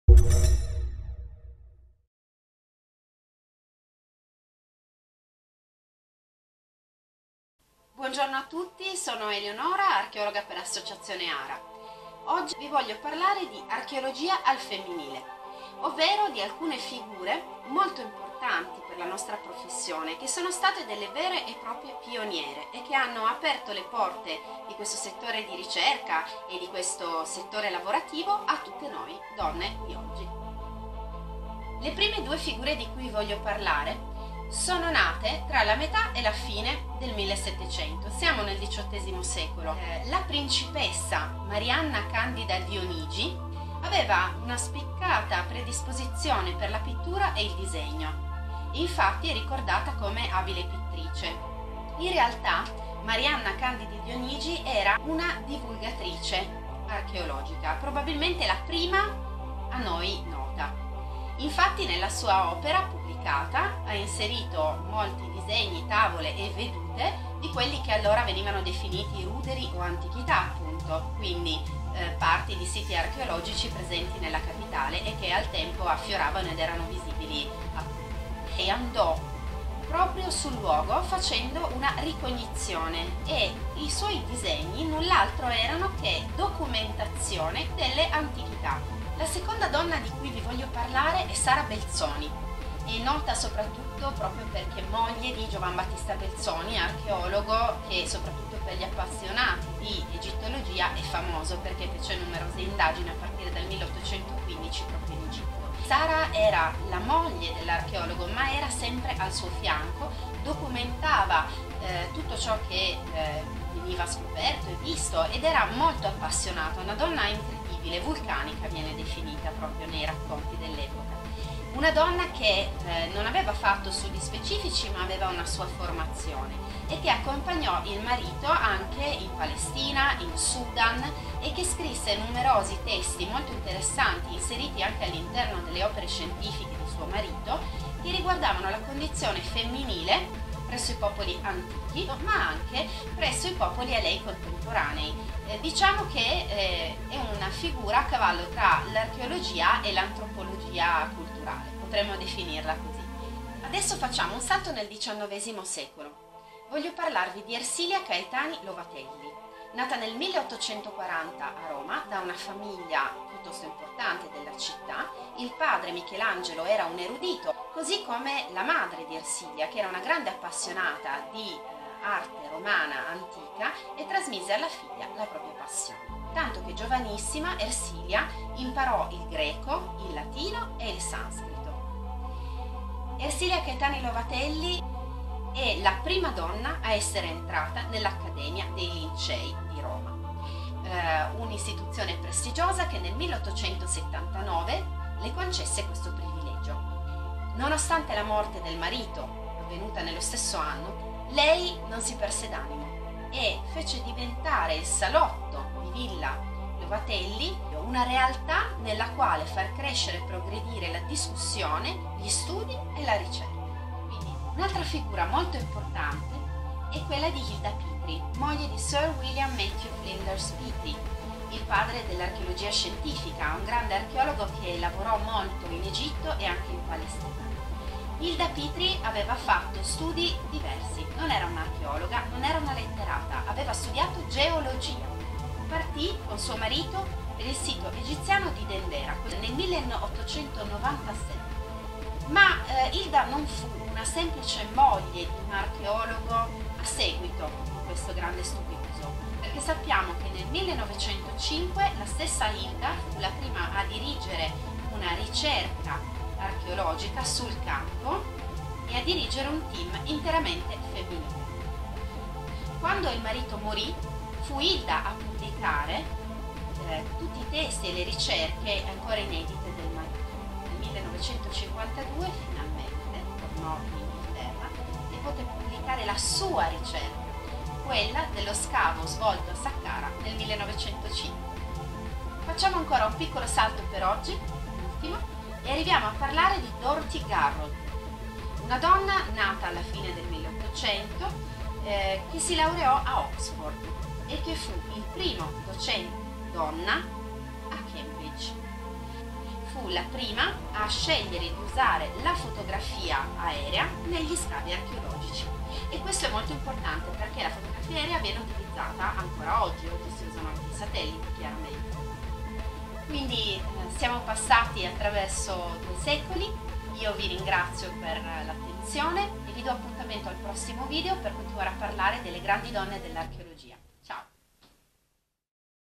Archeologia al femminile - ARA's CULTURAL BOX
Marianna c.Dionigi, Sarah Belzoni, Ersilia C. Lovatelli, Hilde Petrie e D. Garrod sono le pioniere dell'archeologia.
In questo video Eleonora ci racconterà di queste donne, le prime studiose di archeologia, antropologia e preistoria dei nostri tempi.